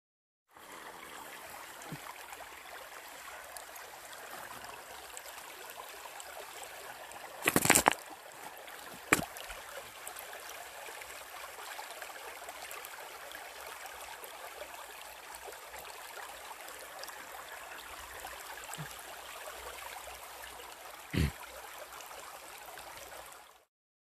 KUMAK GÓRSKI
Jest to mały płaz z rodziny kumakowatych. Dorasta on przeciętnie do 4-5 cm. Zamieszkuje on kałuże, rowy, płytkie zarośnięte zbiorniki wodne, podmokłe łąki, strumyki itp. Prowadzi wybitnie wodny tryb życia. Żywi się bezkręgowcami. Od kumaka nizinnego odróżnić to można po jaśniejszym brzuchu (żólty dla kumaka górskiego, czerwony dla nizinnego) w Polsce spotykamy w górach i na wyżynach.  Najliczniejszy w Bieszczadach i w Beskidach. W Sudetach raczej niespotykany.